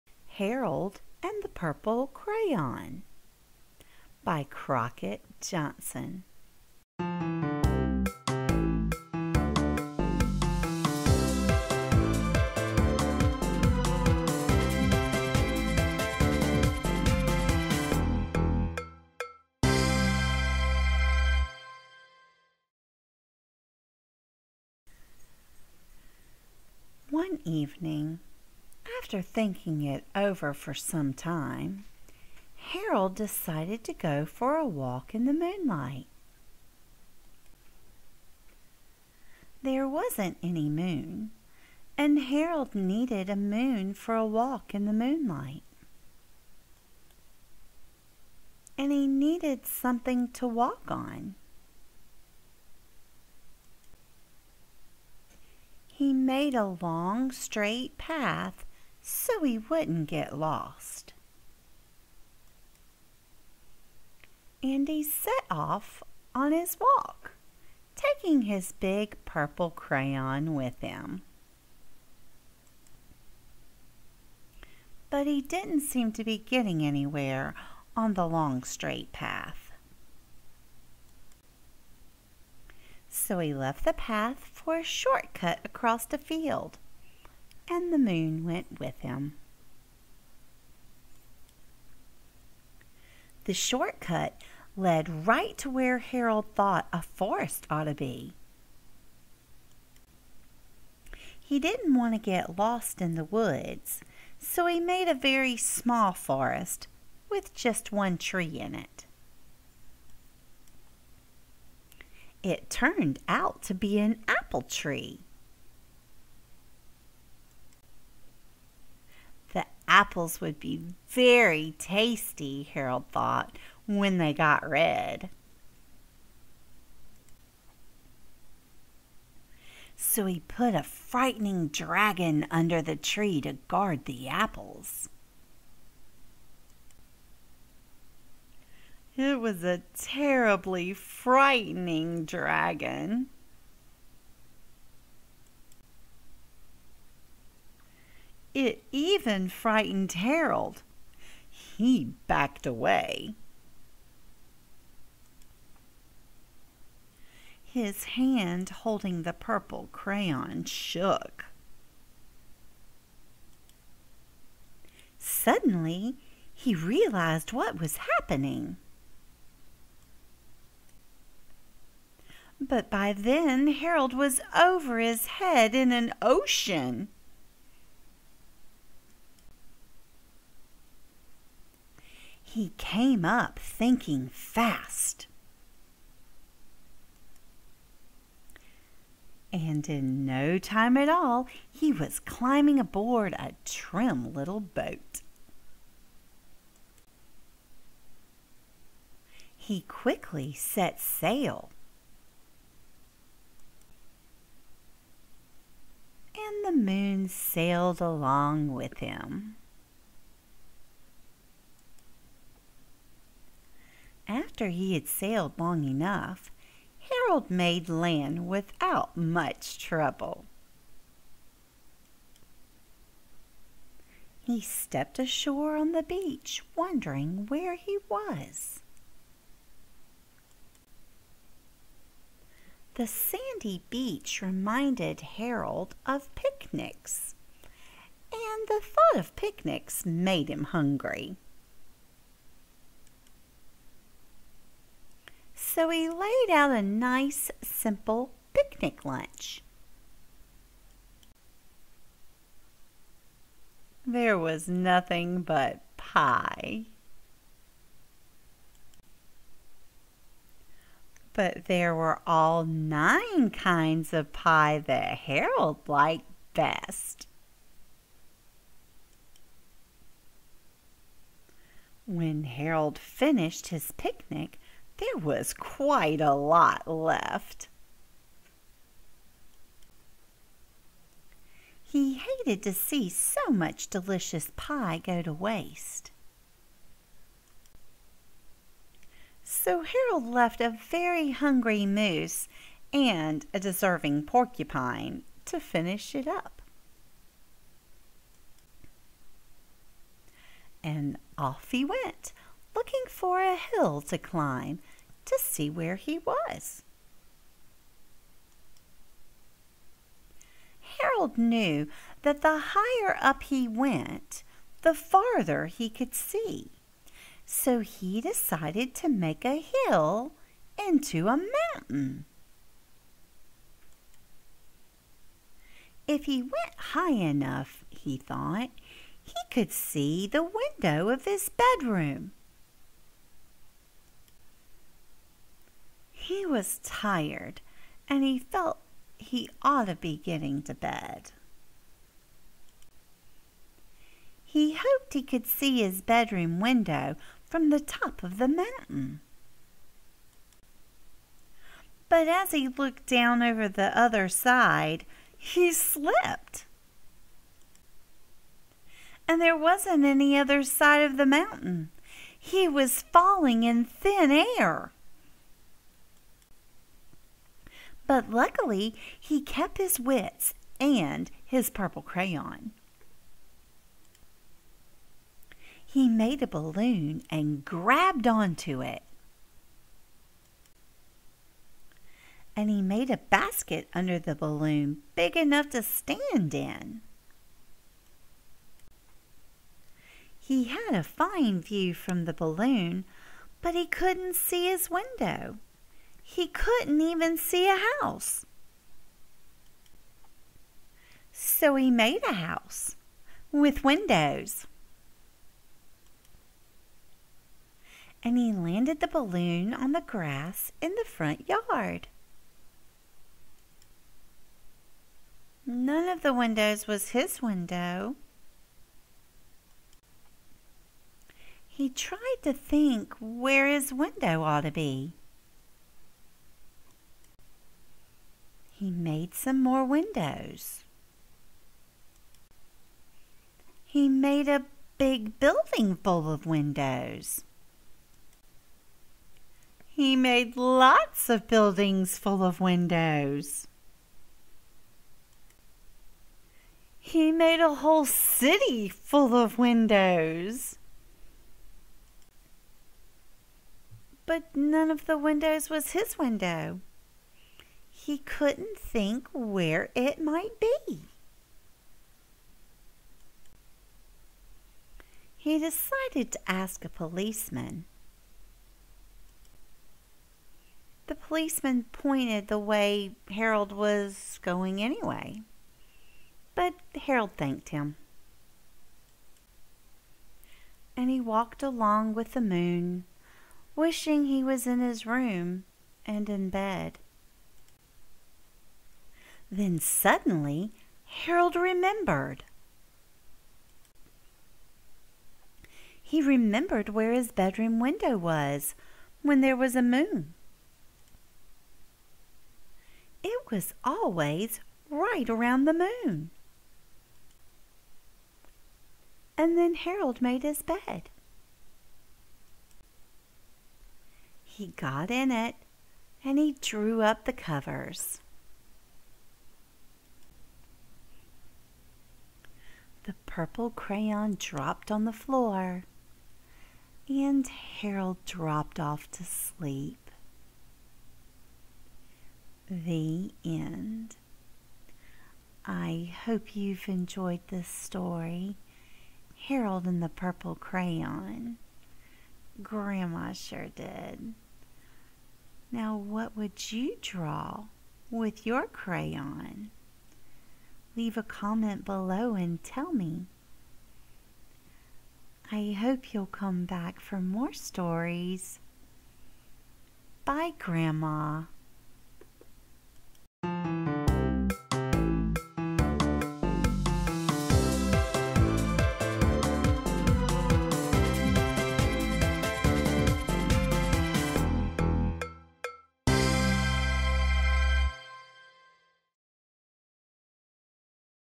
Storytime! ~ HAROLD AND THE PURPLE CRAYON ~ StoryTime ~ Bedtime Story Read Aloud Books
Armed only with an oversized purple crayon, young Harold draws himself a landscape full of wonder and excitement.  Full of funny twists and surprises, this joyful story shows just how far your imagination can take you. Harold and the Purple Crayon has delighted readers of all ages since 1955.

You can also follow Gma on

Gma